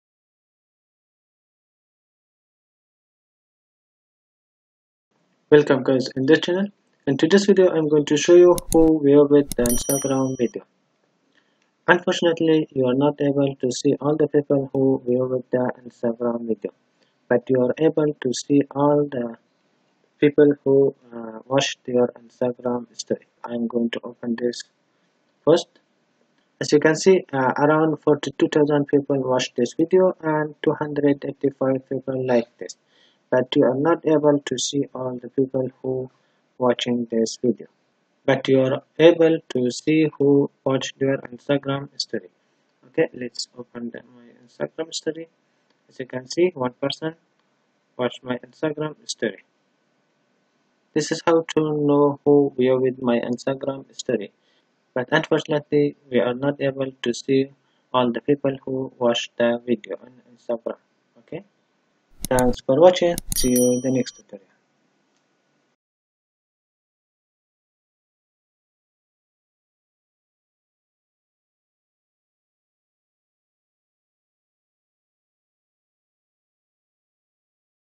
How to see who viewed my video on Instagram 2021
How do know who viewed my Video on the Instagram new feature in the new update? Actually, you are not able to see all people who watched your video on Instagram but you are able to see those who saw your Instagram story.

5. ✅ Search your name on Instagram music and put the first song with your picture on Instagram story